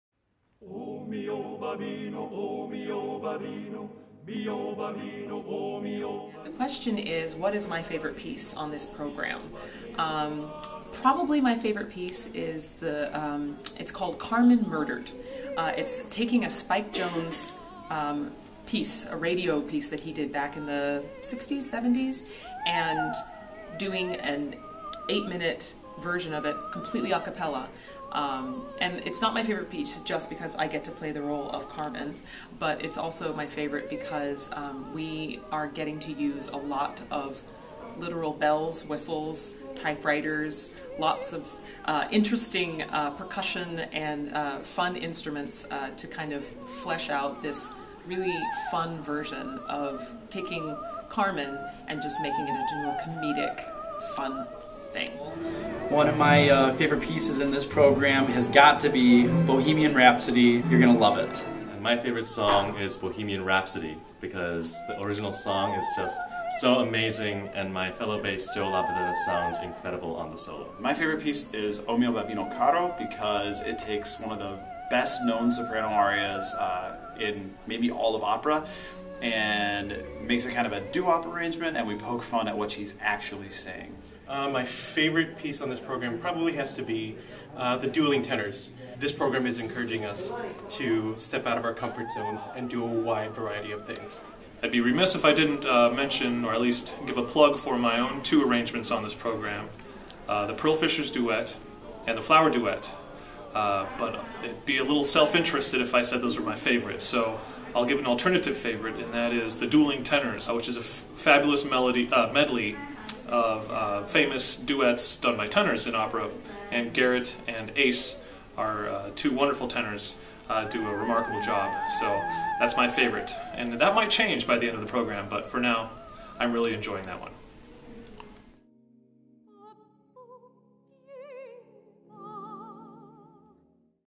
Chicago a cappella talks about: A Night at the Opera
A Night at the Opera performances are April 7-23 in Naperville, Evanston, Hyde Park, and Oak Park. Tickets are on sale now and can be purchased